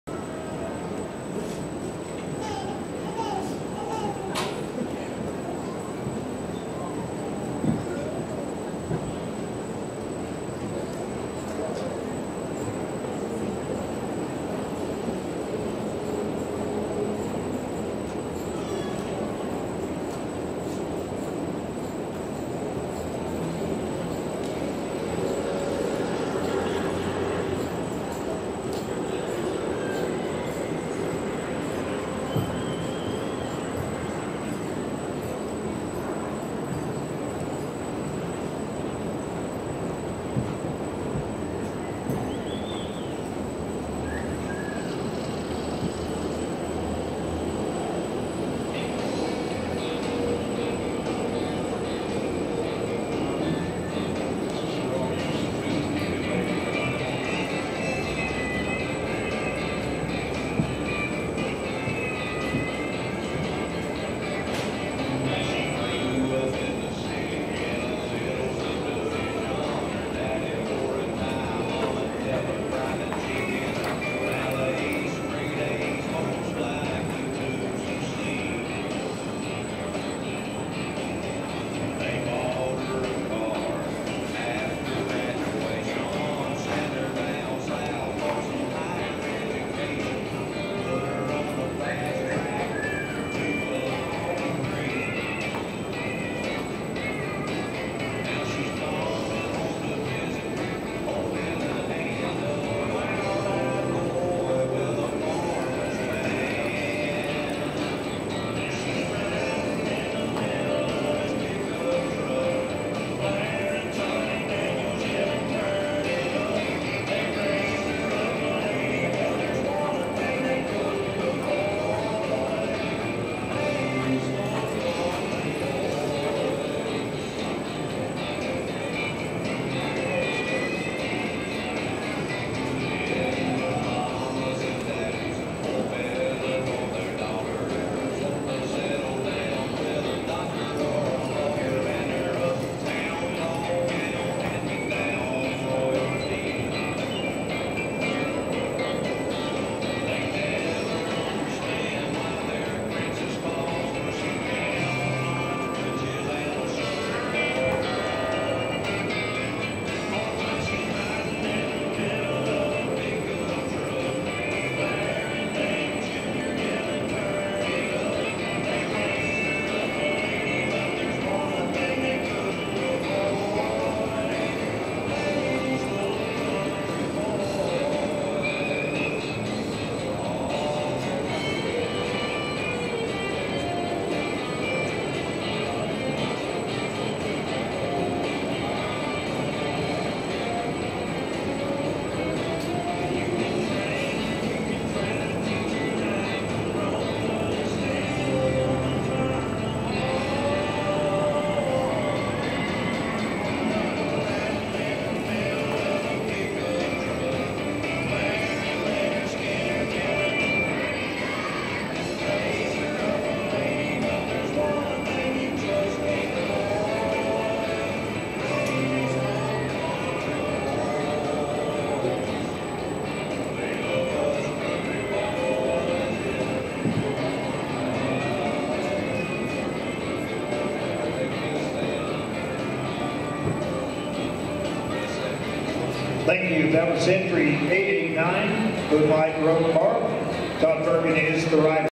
Boonlight Roan Bar Rein
Boonlight Roan Bar shown by Todd Bergen for Tony Grover in the NSHA Open Futurity in Paso Robles, CA, August 2015. Reined Work